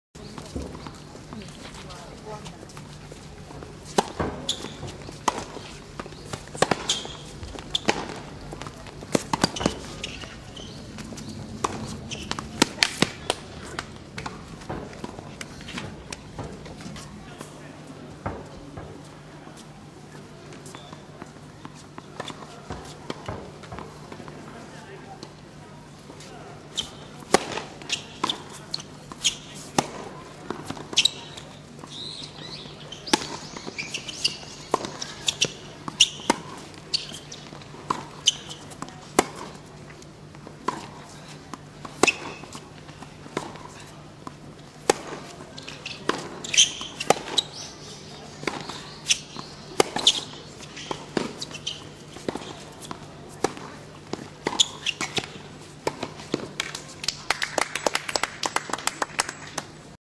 2008 全国選抜ジュニアテニス選手権大会 U14 準々決勝 小堀良太 VS 内田海智
2008 DUNLOP CUP 全国選抜ジュニアテニス選手権大会 U14
男子シングルス 準々決勝 小堀良太 VS 内田海智